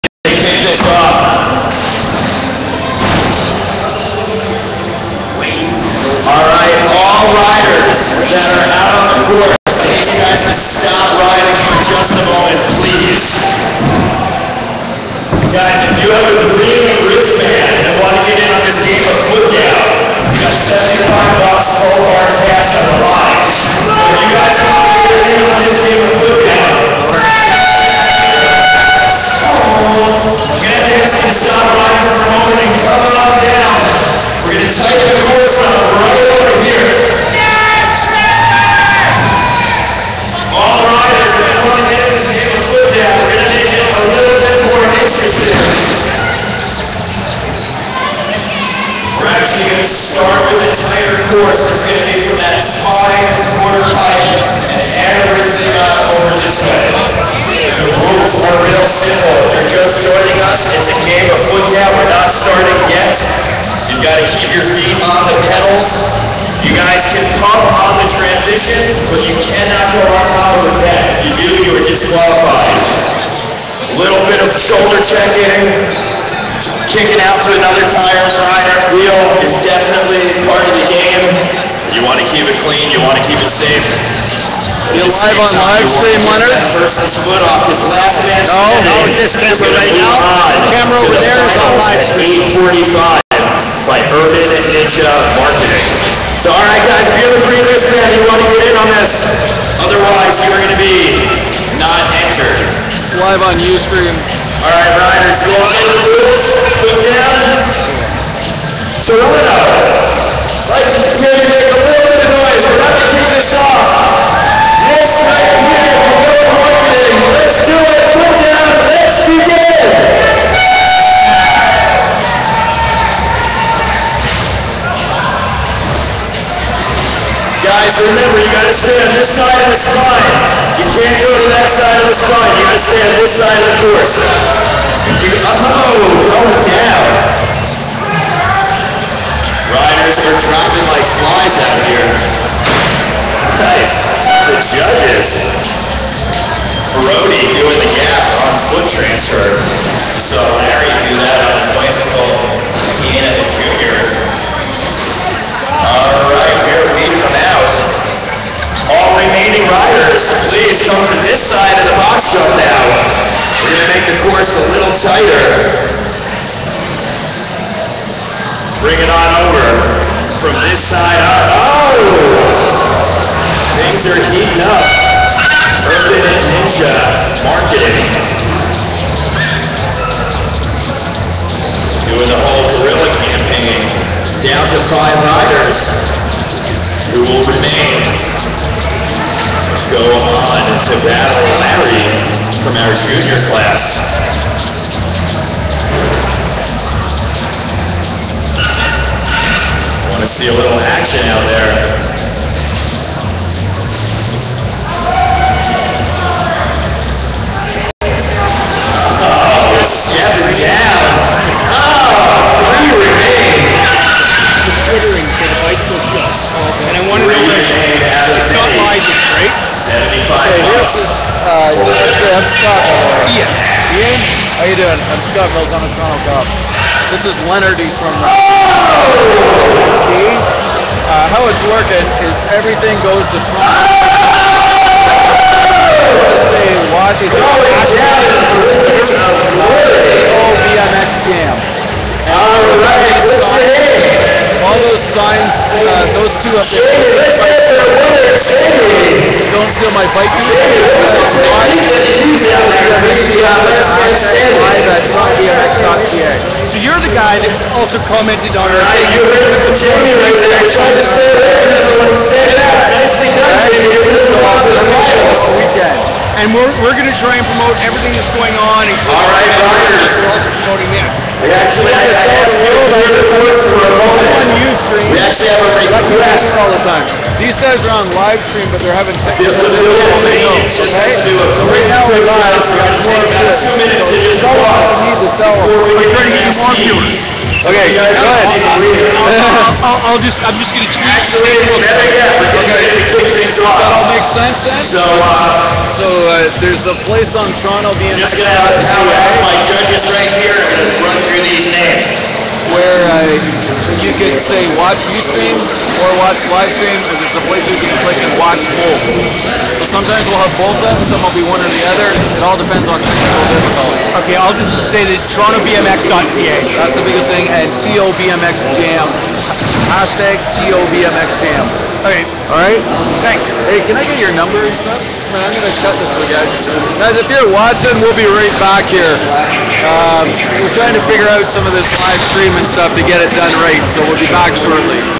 Urban Ninja Guerrilla Marketing Foot Jam
The LAST person still on their bike at the end of the 5 minutes of
madness at the end of each practice session all day FRIDAY and
SATURDAY will head to the foot-down finals.